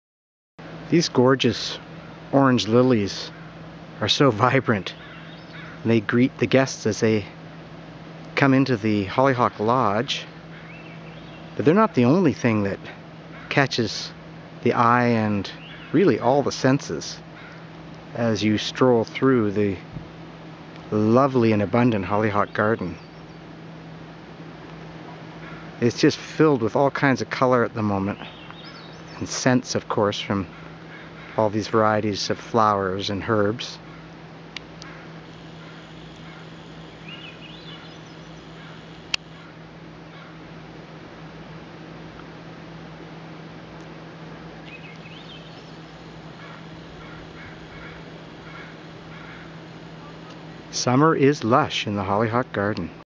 Solstice Garden June 2012
Electric lilyland and more in this mini summer garden tour.